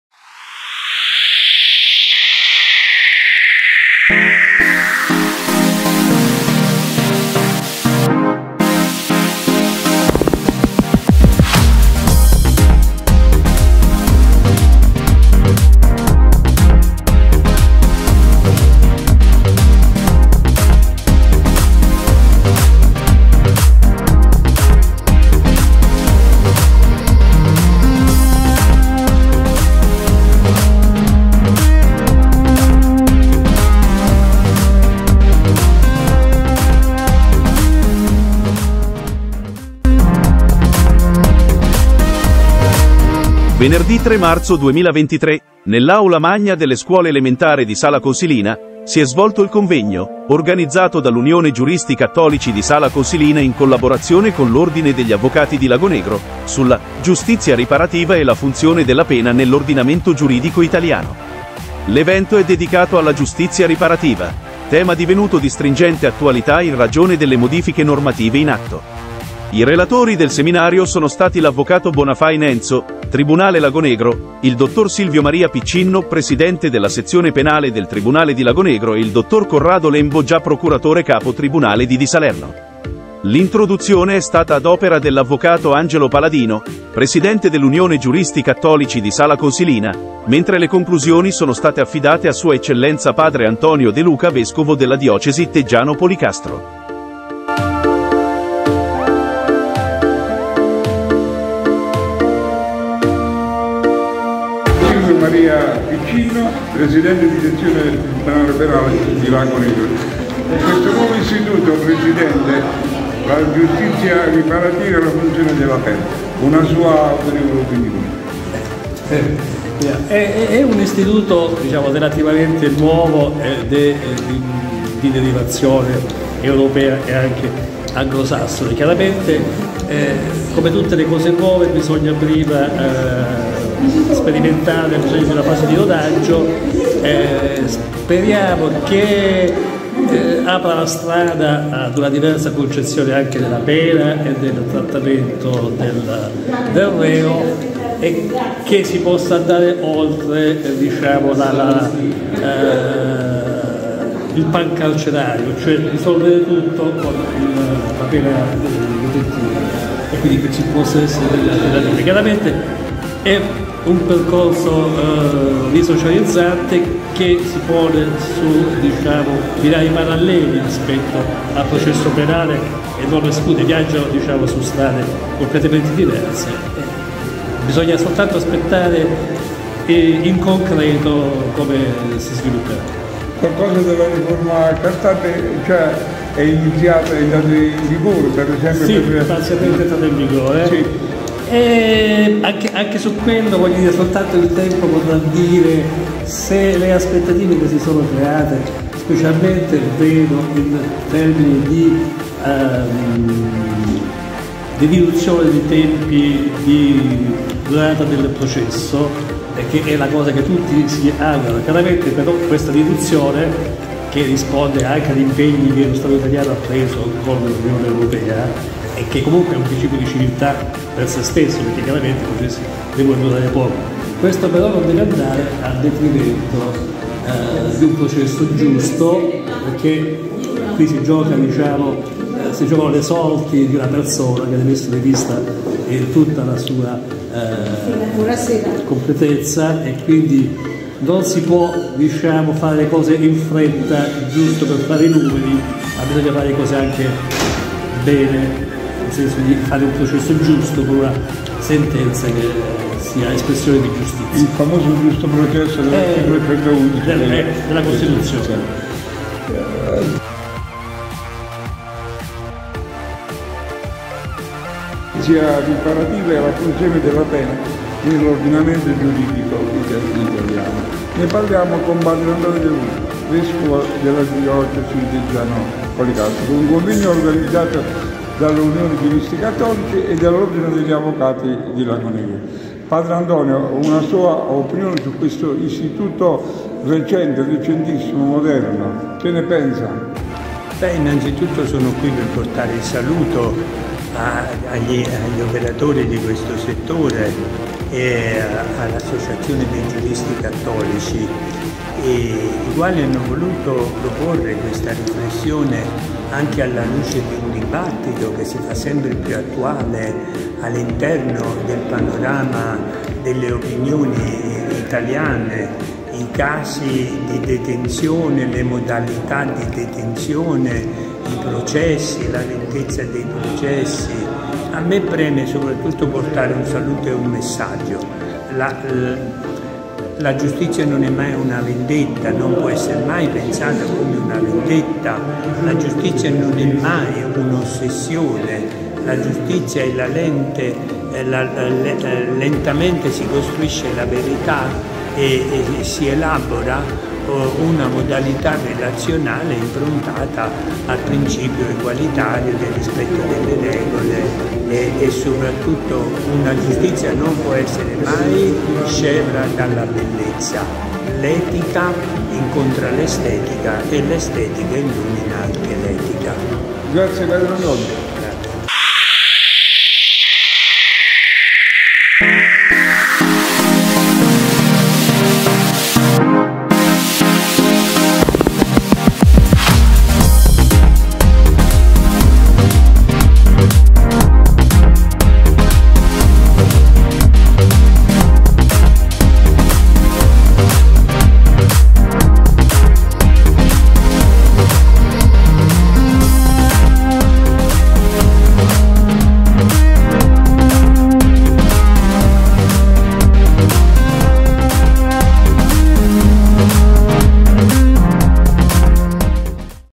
Diano Magazine Convegno del 3 marzo Sala Consilina
LaVoce24 Magazine è un appuntamento settimanale con interviste, fatti, eventi che le nostre redazioni producono per i nostri amici che ci seguono, questa prima puntata è stata realizzata al convegno dello scorso 3 marzo a Sala Consilina sul “Giusto processo”, curata da Pietro Cusati, detto Pierino e Michele D’Alessio, responsabili della Redazione di “LaVoce24 Diano magazine”.